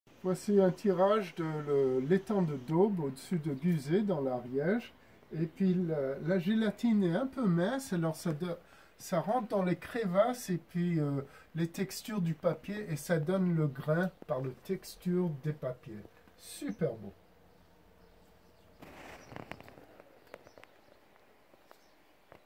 2021 self-made emulsion print of Etang d'Aube in the French Pyrenees.
A very grainy unique print dry today. Made with a new batch of emulsion. Experimenting with gelatin bought in Italy.